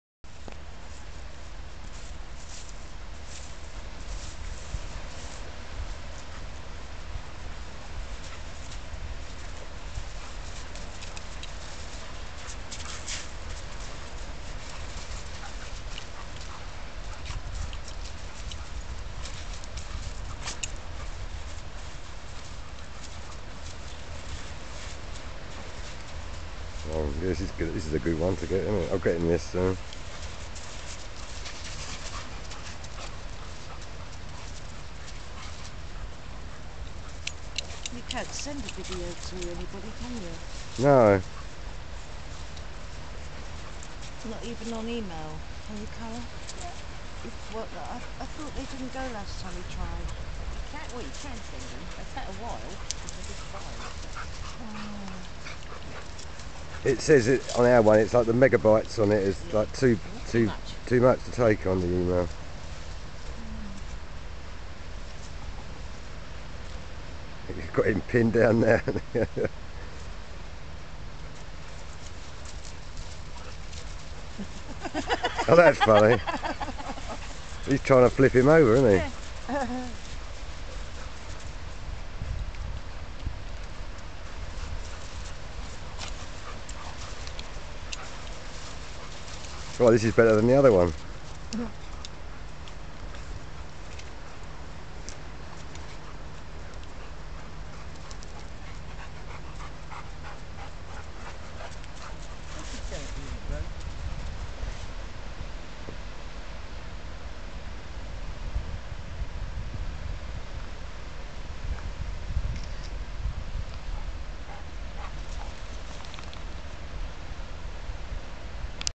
walton on the naze 014
Archie the Airedale with his best mate Bruny having great funj on the beach.